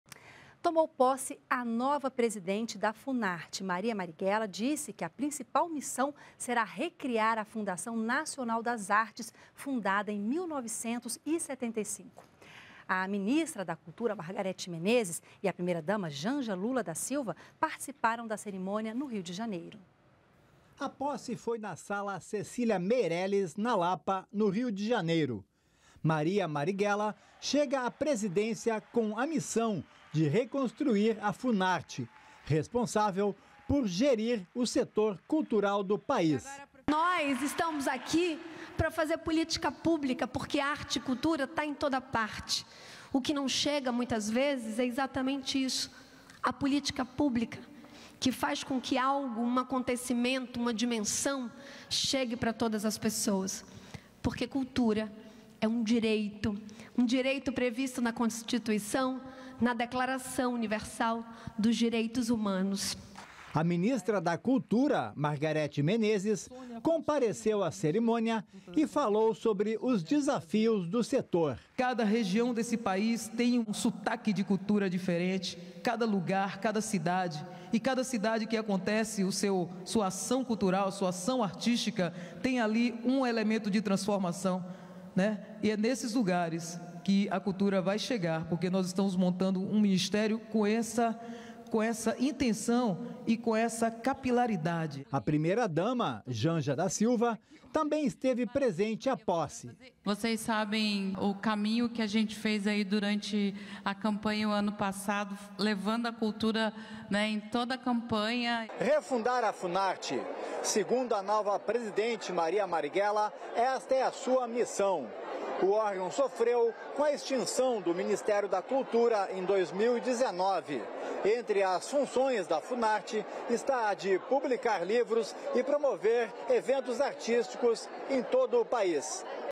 Cultura | Posse na Funarte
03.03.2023 - Maria Marighella tomou posse como a nova presidente da Funarte.